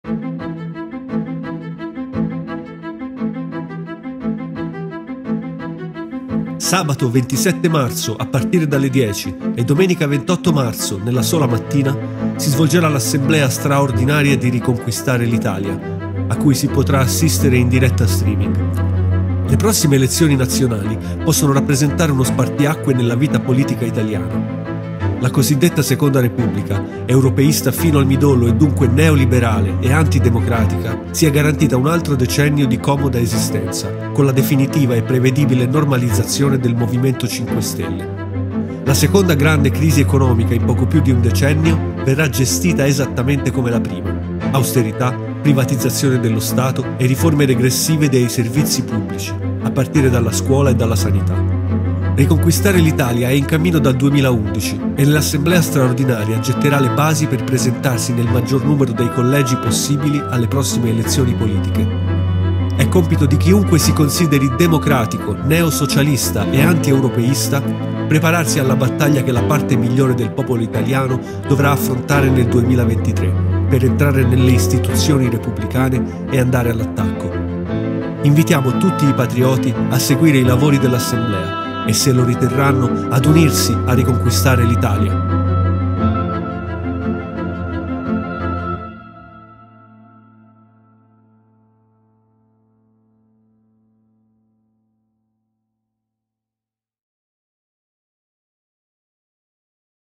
27-28 marzo: Congresso FSI-Riconquistare l'Italia, verso le elezioni 2023 con il Partito del Recesso
Un passaggio fondamentale nella vita del partito, che si proietta verso le elezioni politiche del 2023, nelle quali sarà fondamentale riuscire ad esprimere validi candidati in tutti i Collegi d’Italia: subito dopo l'Assemblea (che quest'anno sarà Congresso) inizieremo a lavorare in questa direzione.

Chiunque si consideri democratico, neosocialista e anti-europeista, potrà dare il proprio contributo, unendosi a Riconquistare l'Italia: è prevista l'introduzione della figura dei soci simpatizzanti, con limitatissimi diritti e doveri, ma con diritto e dovere di partecipare alla campagna elettorale del 2023.

Sarà possibile assistere al Congresso in streaming su questo canale YouTube.

📝 Per l'iscrizione al Partito e la partecipazione come soci al Congresso basta compilare questo form